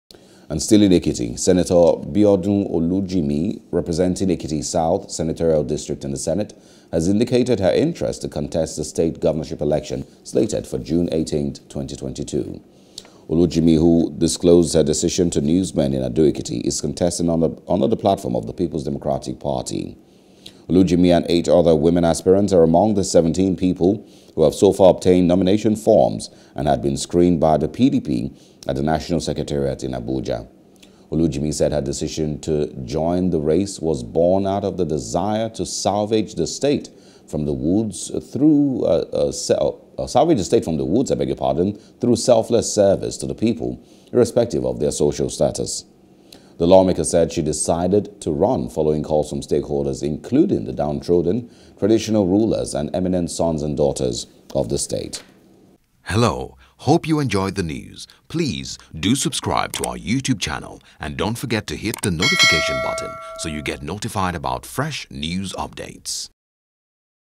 Ekiti 2022: Sen. Olujimi, 8 Other Women Join Race | NEWS
Still in Ekiti, Senator Biodun Olujimi, representing Ekiti South Senatorial District in the Senate has indicated her interest to contest the state Governorship election, slated for June 18, 2022.